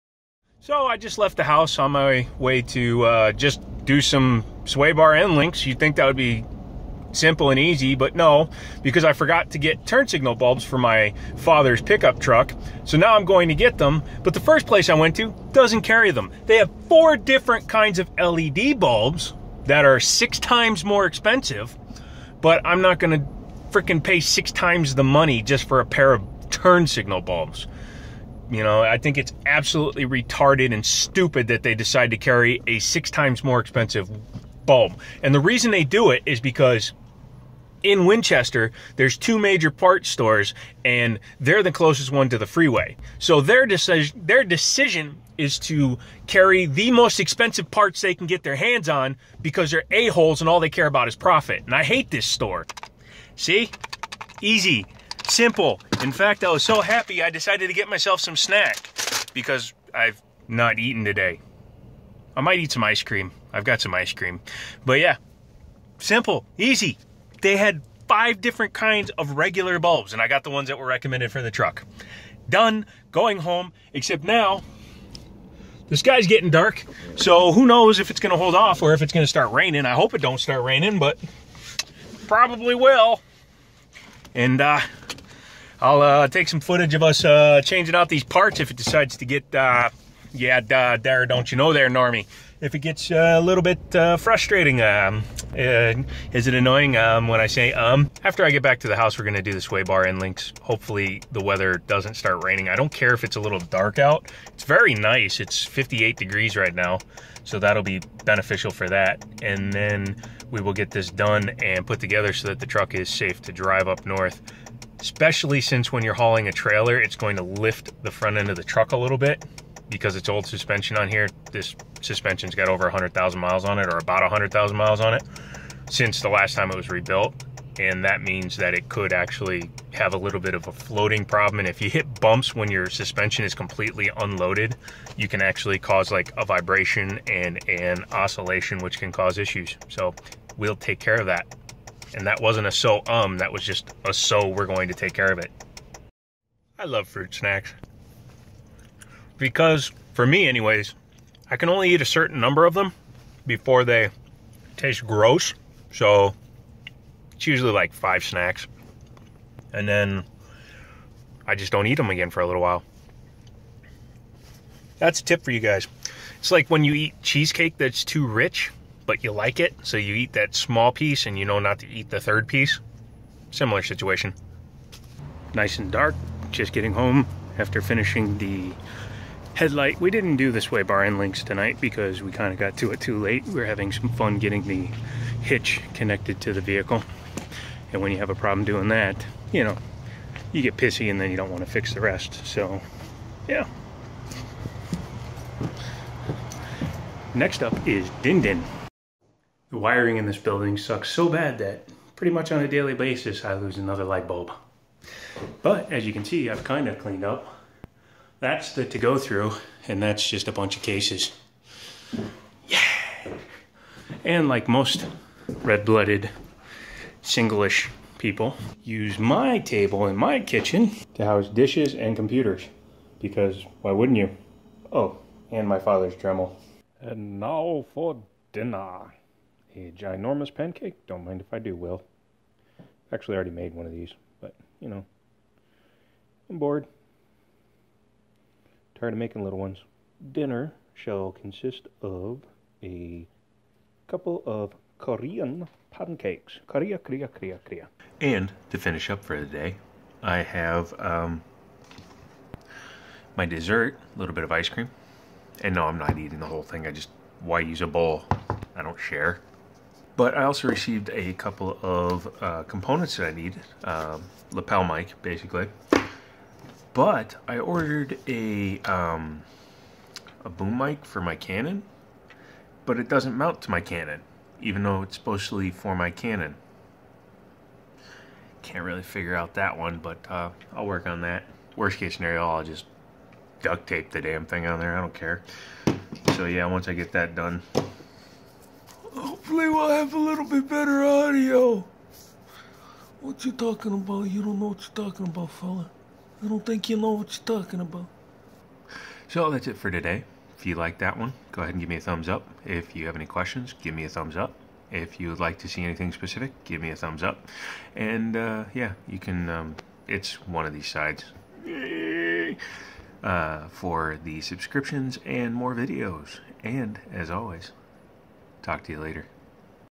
$16 Food Challenge Day 7
Finally eating again. Here is a little Vlog of my day including helping out my Father